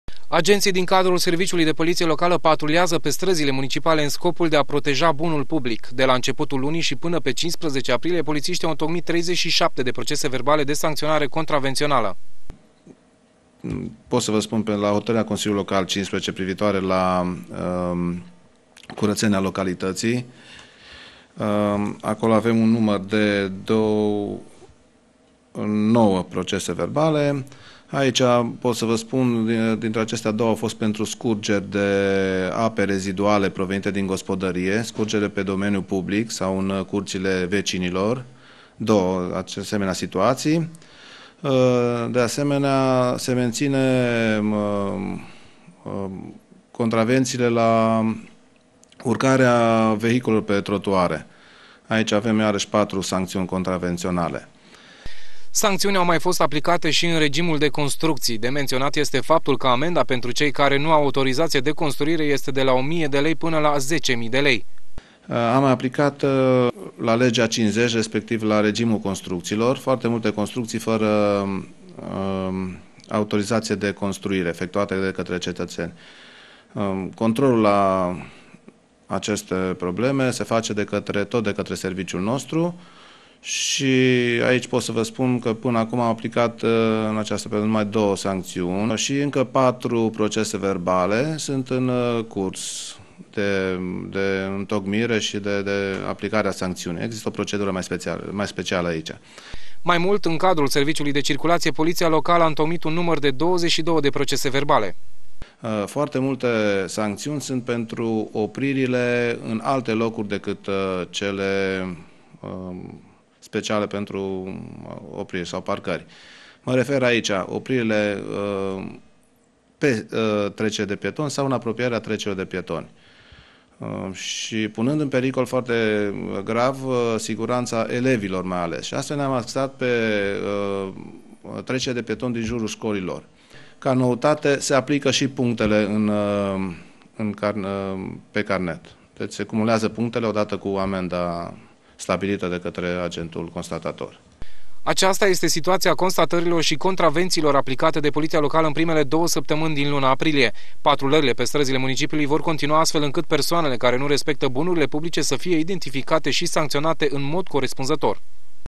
SANCŢIUNI ÎN APRILIE
Poliţia Locală a întocmit în prima jumătate a lunii aprilie peste 50 de procese verbale de sancţionare contravenţională. Acestea au fost atât pentru scurgerile de ape reziduale pe domeniul public, cât şi pentru construcţii fără autorizaţie sau parcări pe trecerile de pieton.